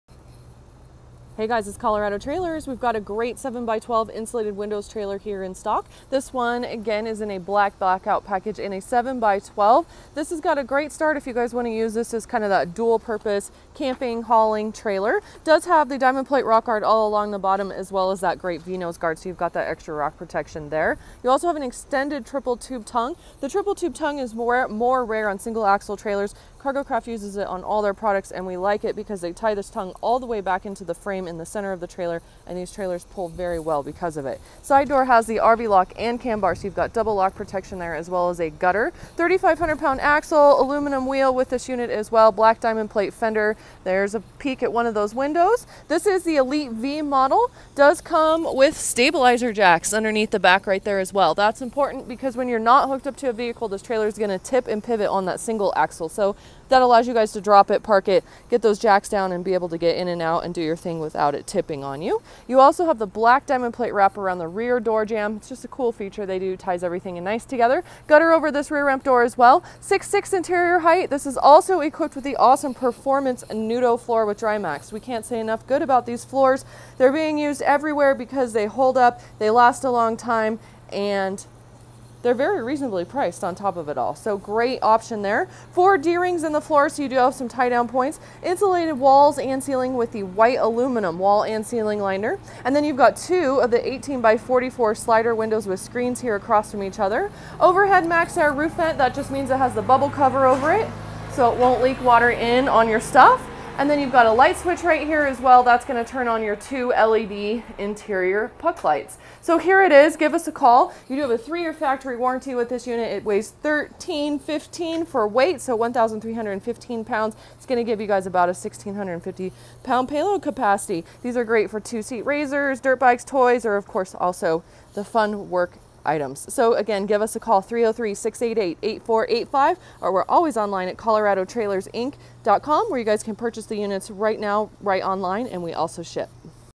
Beautiful Black Blackout 7x12 Insulated Cargo Trailer for sale - Colorado Trailers
New 2020 Cargo Craft 7x12 Insulated Cargo Trailer for sale.  Trailer features:

3500lb. axle
Aluminum wheels
Rear ramp door with spring assist close
Side door with RV lock and cam bar
6 foot 6 inch interior height
Blackout package
Nudo floor and ramp
D-rings
Insulated walls and ceiling with aluminum wall and ceiling liner
Maxx Air roof vent
2 interior LED puck lights with wall switch
LED exterior running lights
Stabilizer jacks
Triple tube tongue
Dealer Stock #15315
Year: 2019
Make: Cargo Craft
Model: 7x12
Color: black blackout
Weight: 1315 lbs.
Shipping options available with great rates! Call to inquire.